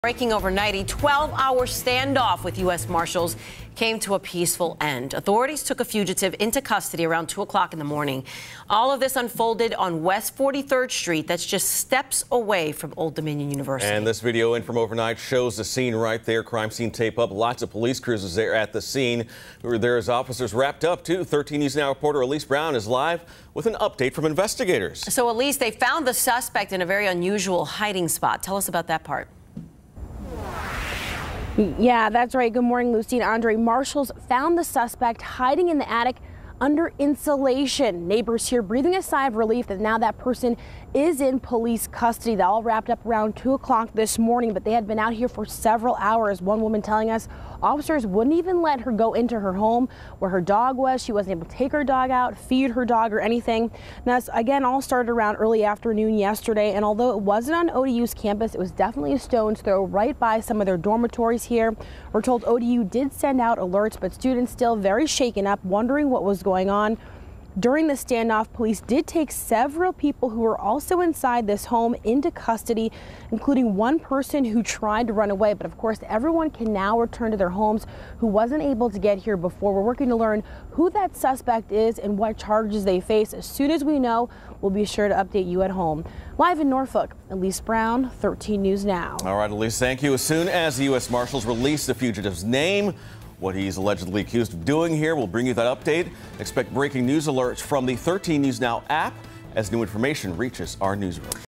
12-hour standoff ends with fugitive's arrest near ODU
12-hour standoff ends with fugitive's arrest near ODU